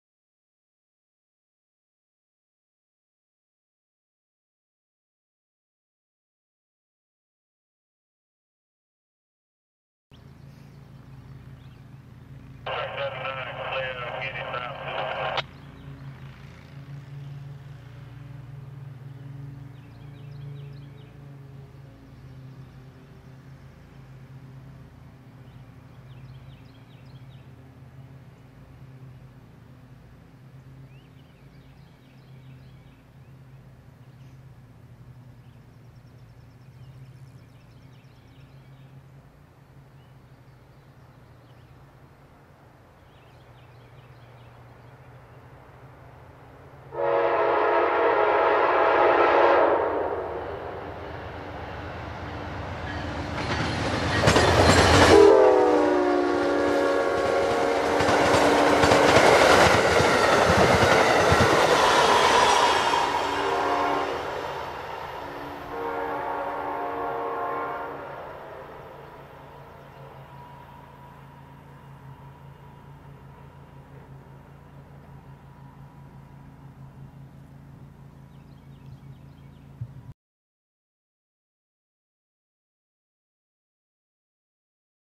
Amtrak Carolinian, train 79, Guinea, VA 4 29 15
Finally was the south bound Carolinian, train 79, with P42 188 and a NCDot coach Frazier Fur on the rear. After he was by decided to get back on No. 1 and head home with a run by Ashland on the way.